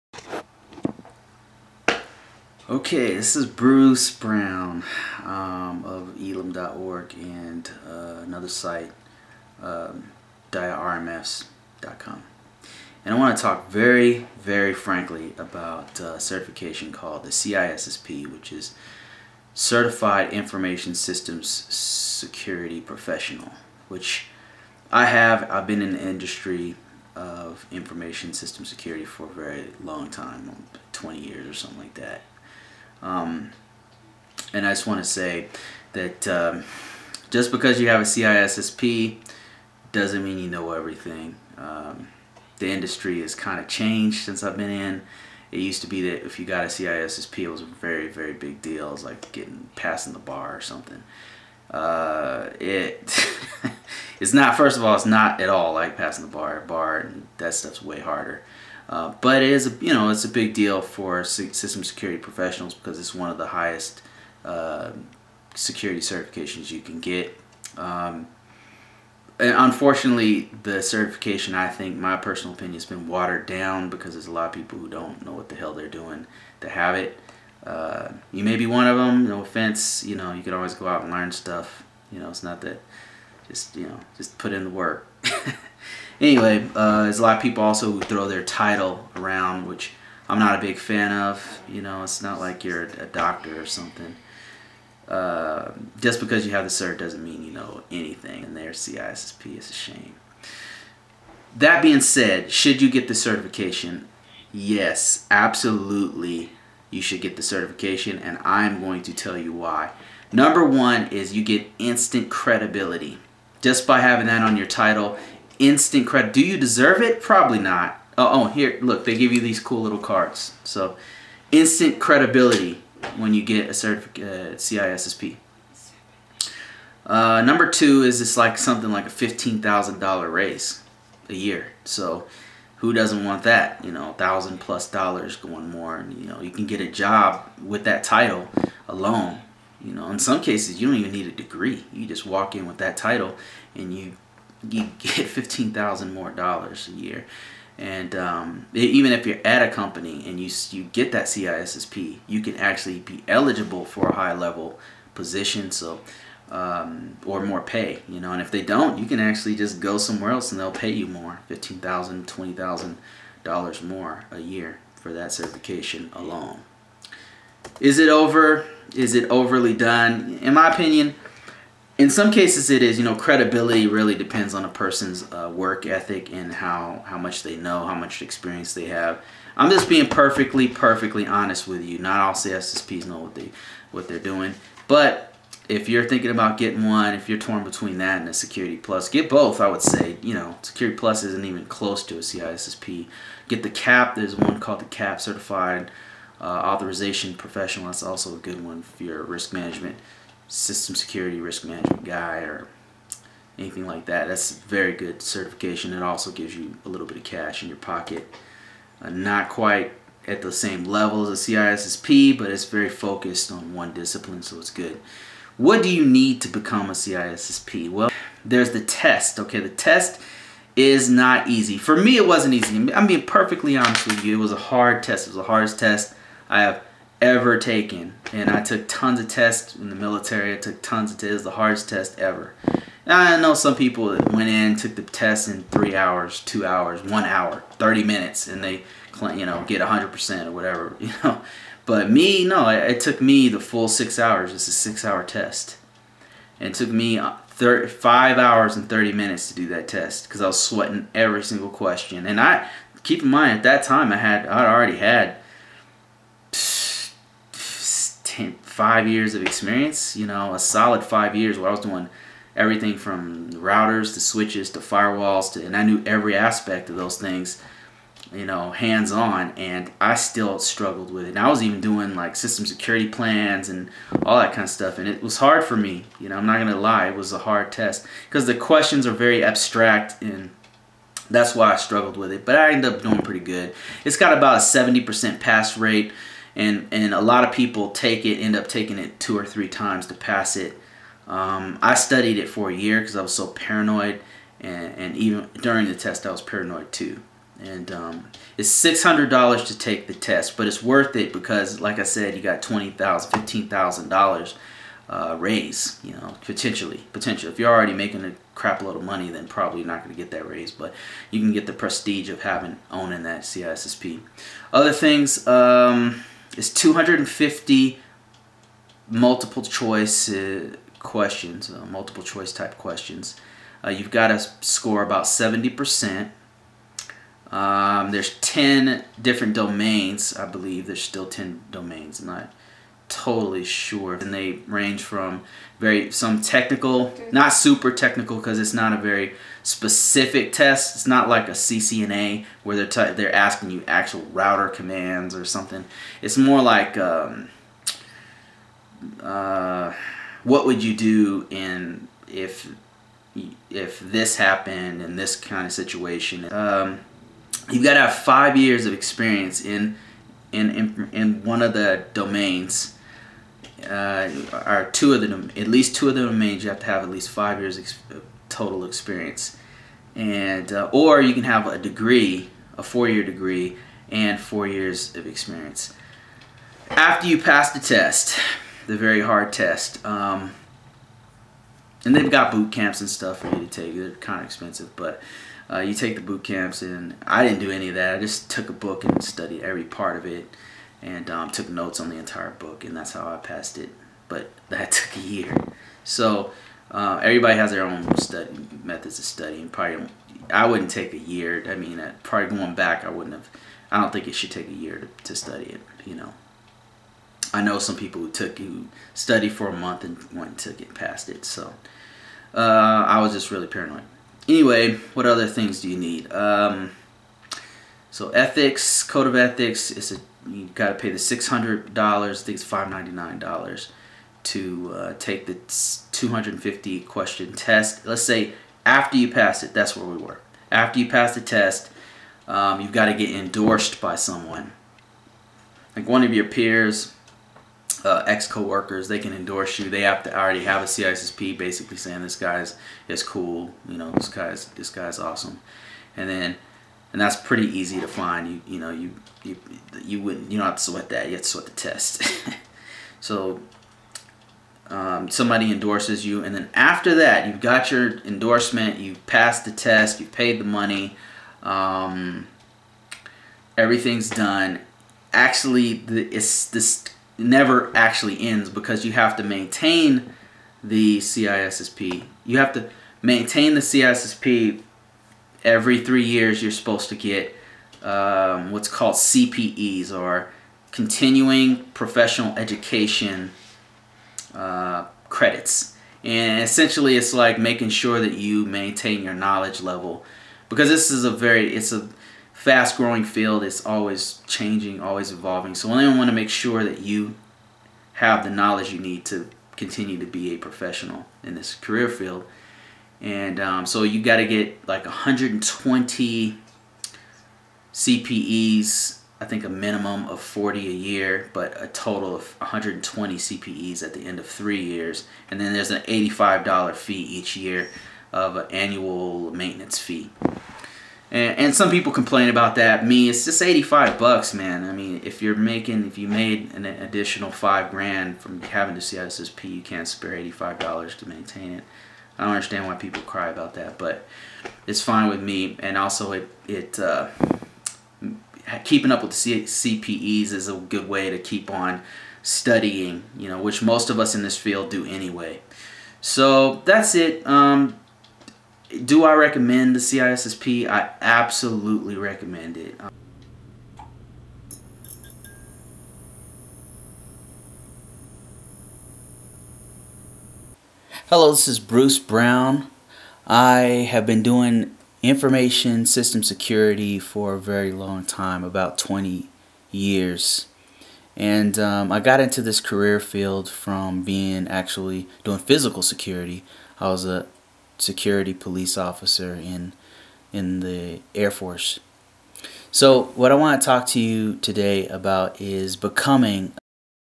become a cissp
I got the ISC2 Certified Information System Security Professional (CISSP) cert in 2006.

Here is my take on the information system security field and the CISSP.

CISSP Requirements:
Candidates must have a minimum of 5 years cumulative paid full-time work experience in two or more of the 8 domains of the (ISC)² CISSP CBK®. Candidates may receive a one year experience waiver with a 4-year college degree, or regional equivalent or additional credential from the (ISC)² approved list, thus requiring four years of direct full-time professional security work experience in 2 or more of the 8 domains of the CISSP CBK.

Don't have the experience? Become an Associate of (ISC)² by successfully passing the CISSP exam. You'll have 6 years to earn your experience to become a CISSP.

Once you have received your (ISC)² email informing you that you have passed the exam, you will need to download and submit the Candidate Endorsement Form.

The form must be completed and signed by an (ISC)² certified professional. The (ISC)² certified professional is anyone who:

Is able to attest to your professional experience
Is an active (ISC)² credential holder in good standing